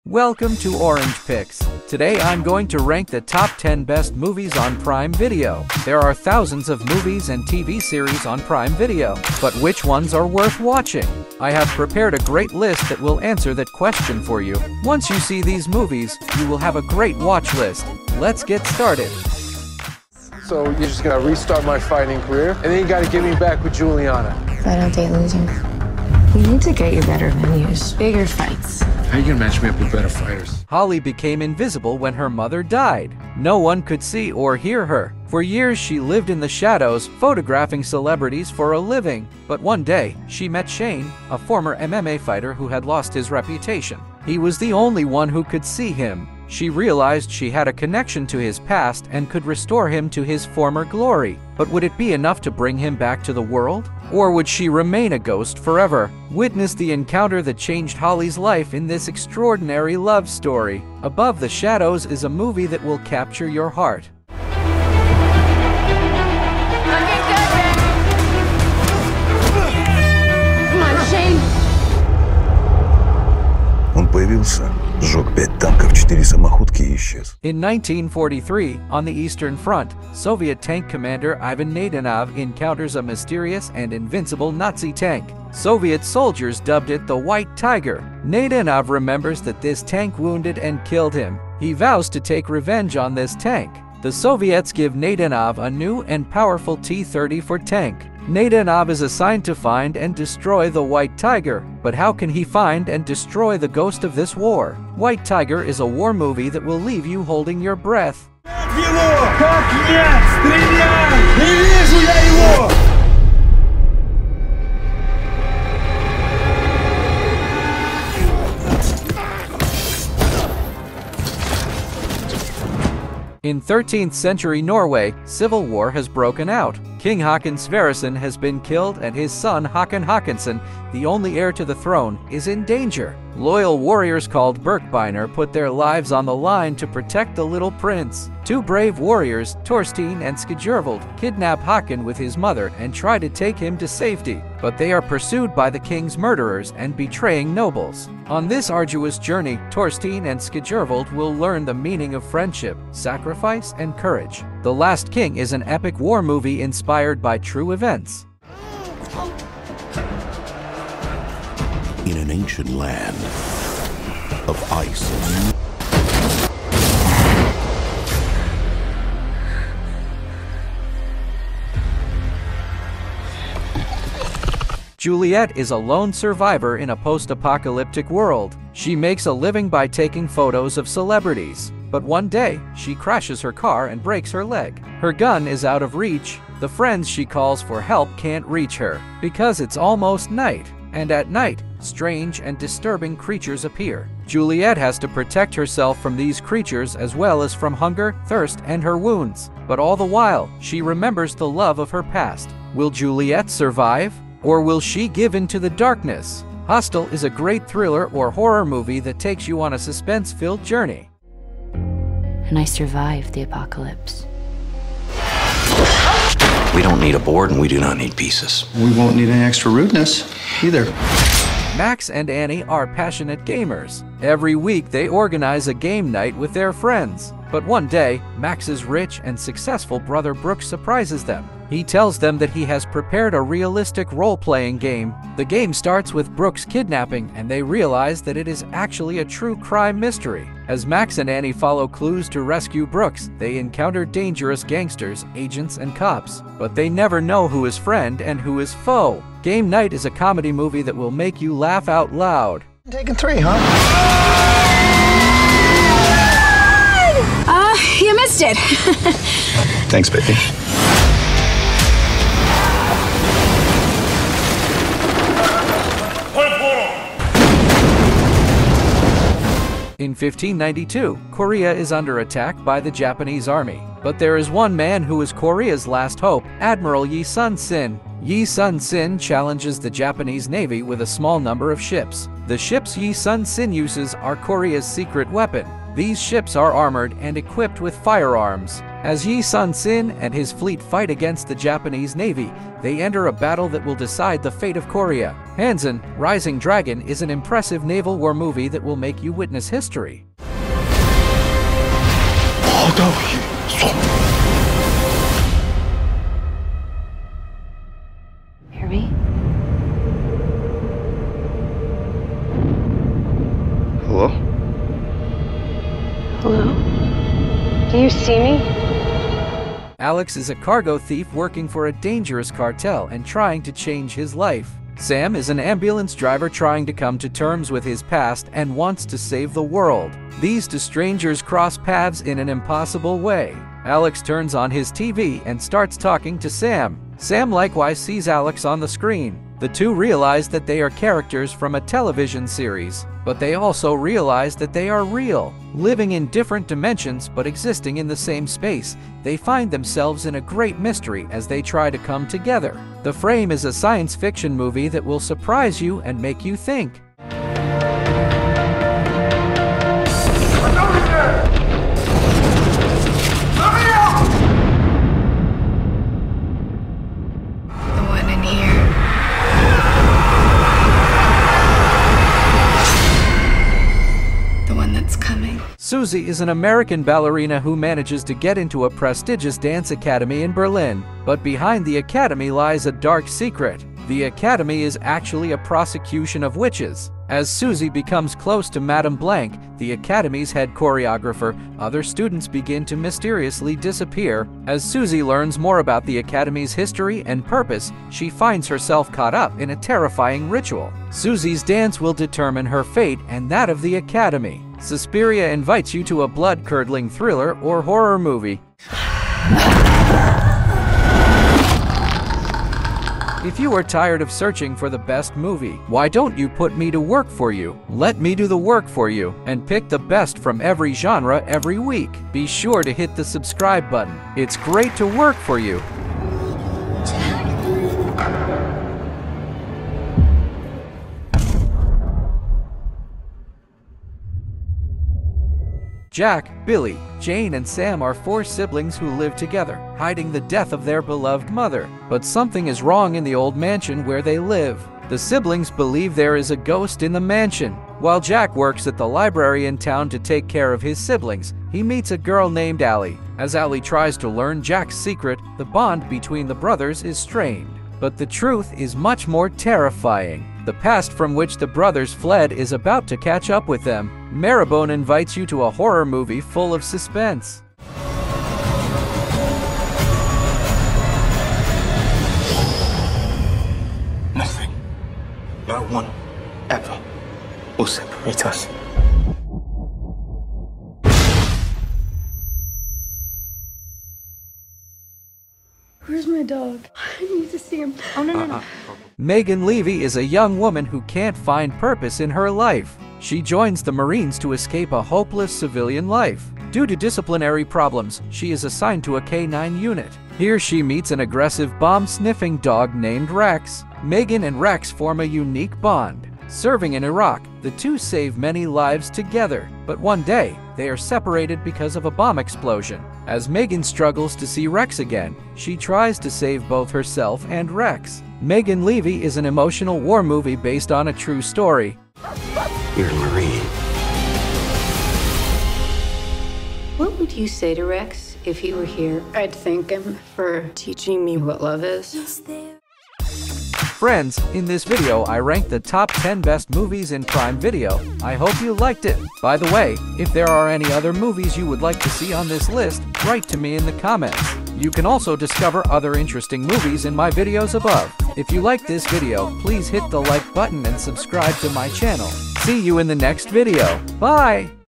Top 10 Best New Movies On Amazon Prime Video to Watch NOW! Dominating Netflix in 2023!
Hello friends, today I am recommending you the top 10 best movies you can watch on Prime Video. These movies are so good and high quality that they make Prime Video better than NETFLIX. Comedy, drama, action, thriller, horror, science fiction... You will find a movie for every taste. It was very hard to rank these movies, but I tried my best to do it for you. Here are the 10 best movies on Prime Video!